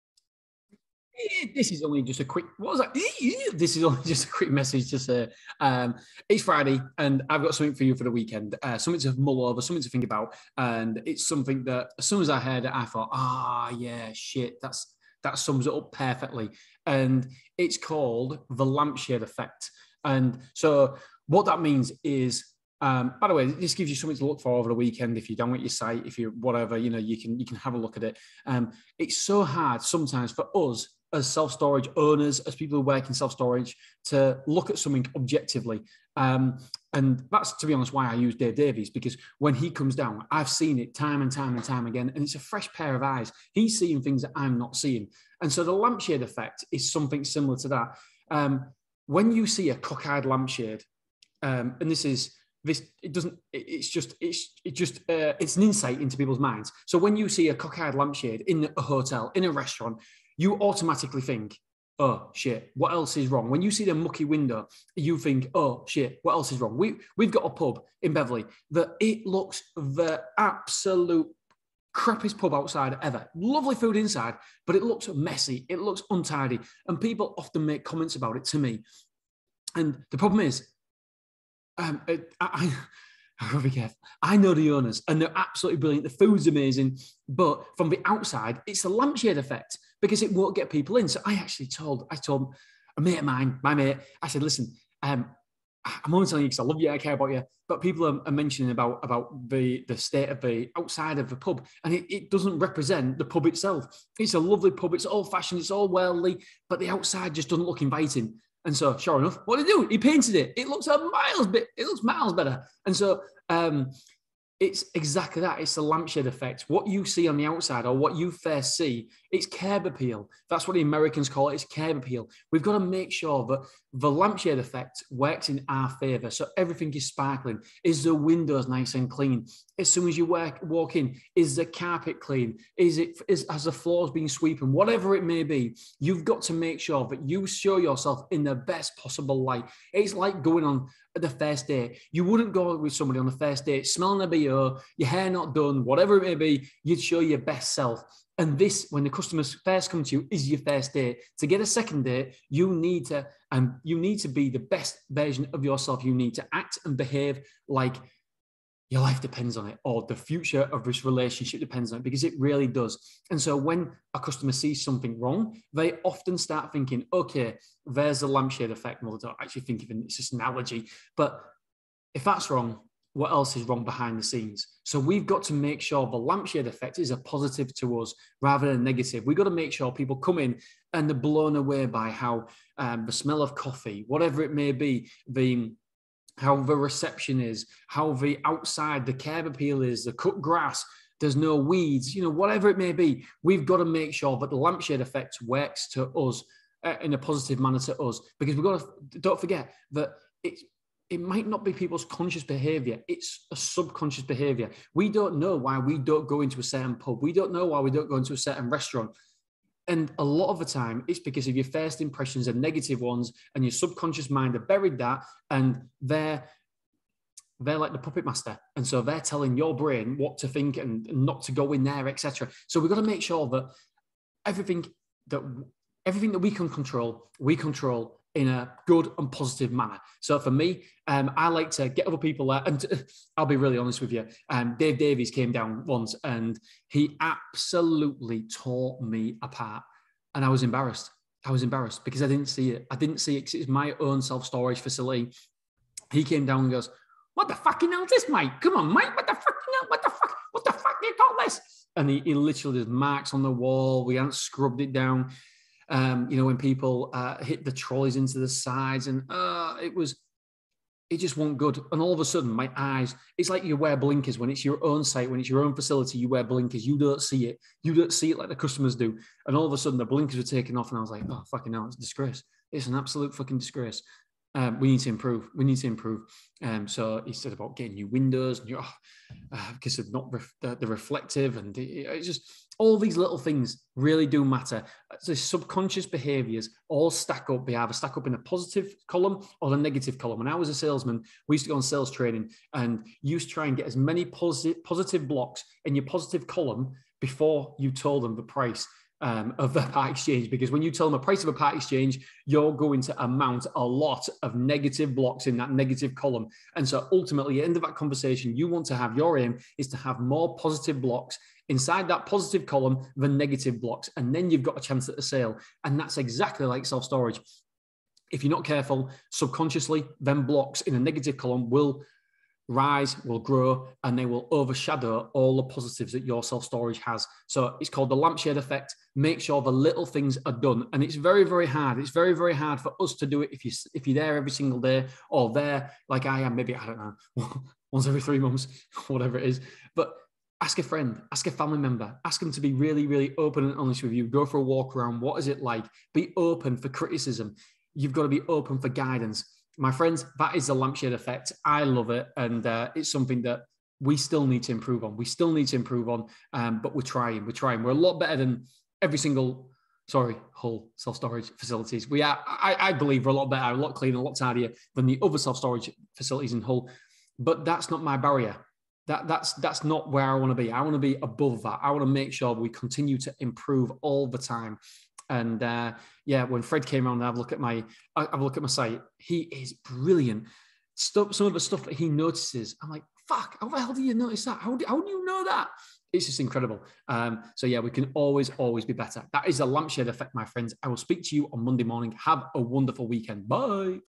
#363 - The Lampshade Effect
Good morning Store Nation!

Welcome back to the podcast.

I'm your host, Dean Booty.

Today is Friday and we are talking about The Lampshade Effect.

The tendency for people to take a small error or oversight in the customer experience and infer from it that something bigger may be amiss.

Hope you gain some new knowledge and insight from this episode.

Please give a listen.

Andy of Approved Business Finance: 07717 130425